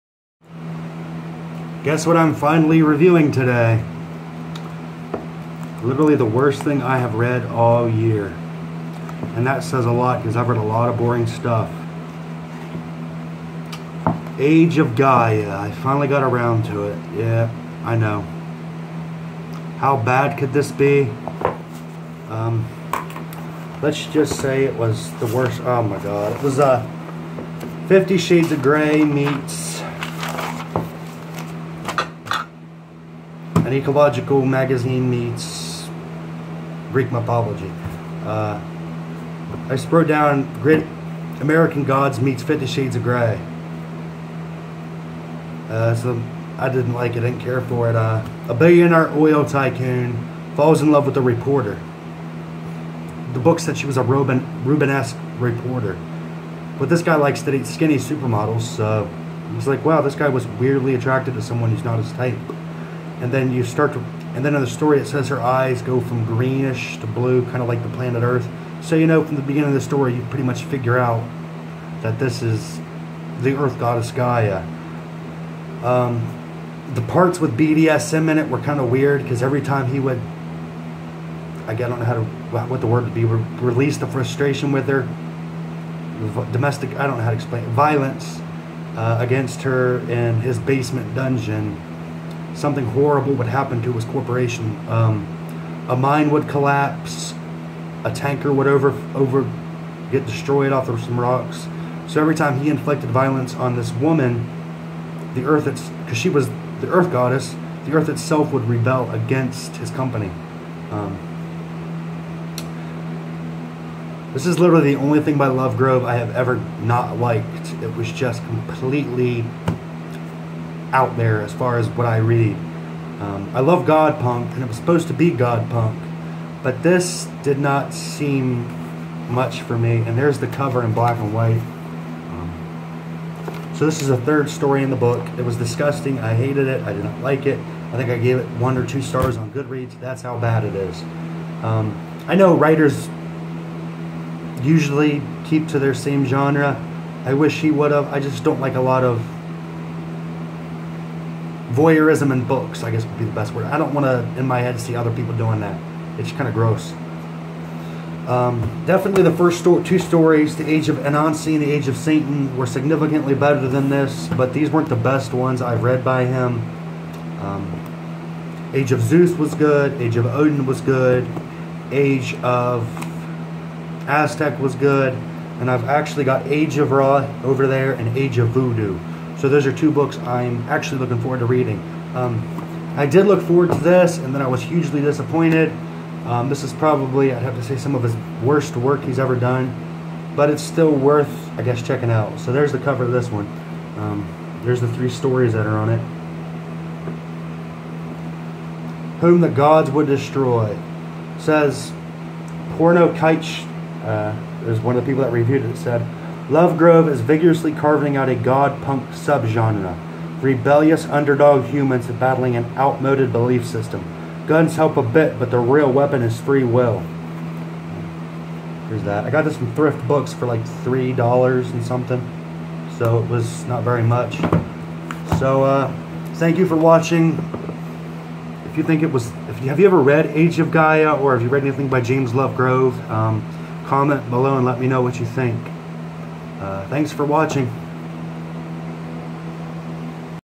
Age of Gaia (Rant-Review )
This Novella took me way out of my Comfort Zone. Which I guess in someways in the Point of Reading . I Gave it One Star and Tried Not To Give away you many Spoilers